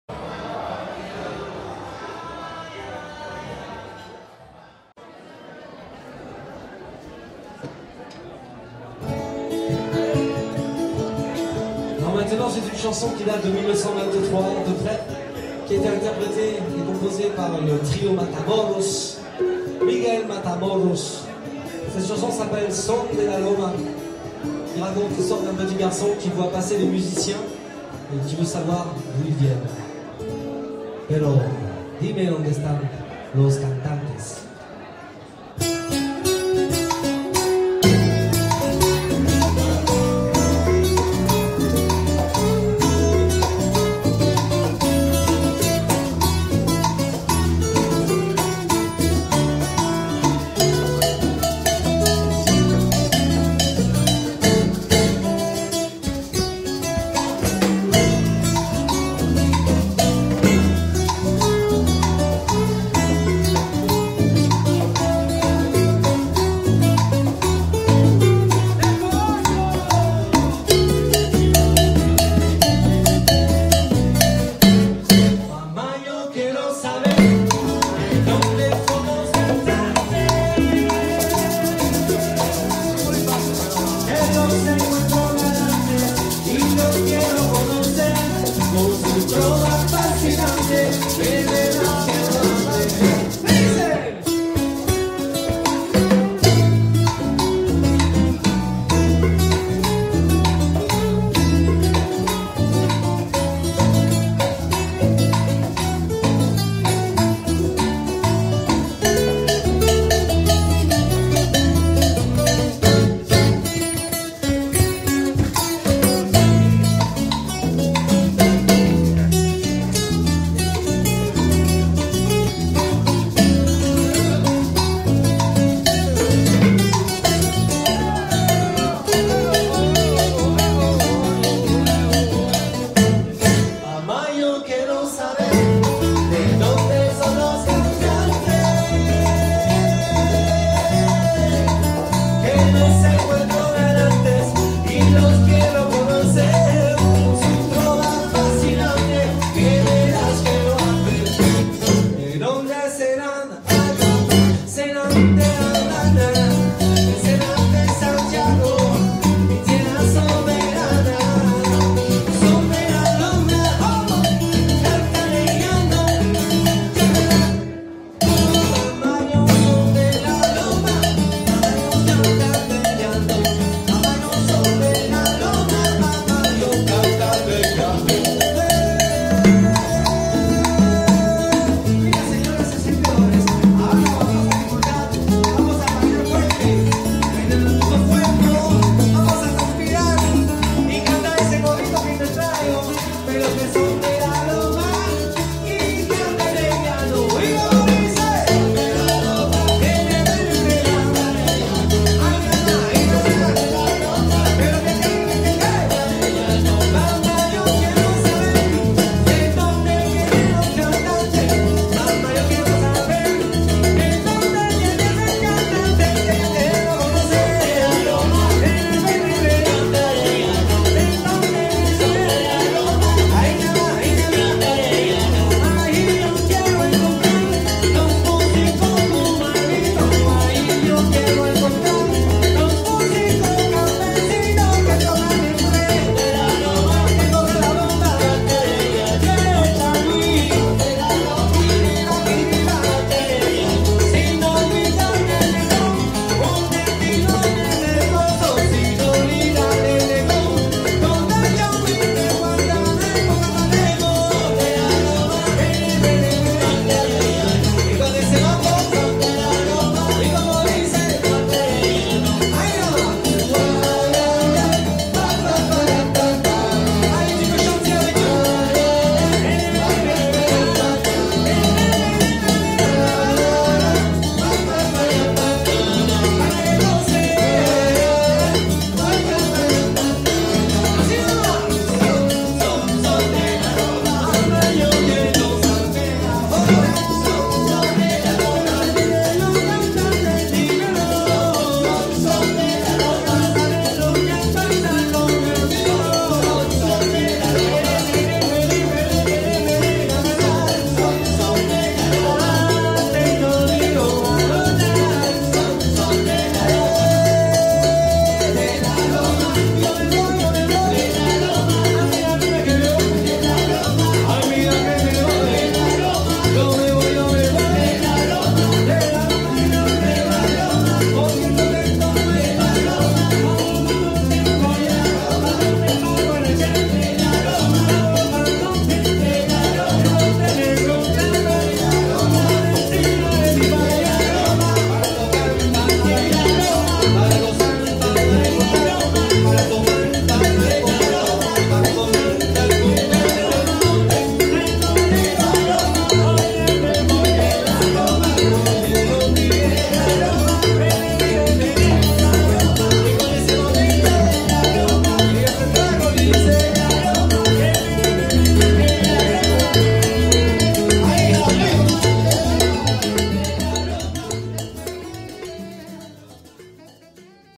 CONCIERTO EN EL MOULIN GAZAY : CONJUNTO JALEO ⎢SALSA DURA⎢
Film de la soirée salsa avec le groupe Conjunto Jaleo de Montpellier au Moulin Gazay à Nîmes le 21 octobre 2011.
Organisation : Salsa Esperanza
Réalisation : FB de TIEMPOVIDEO pour salsamag34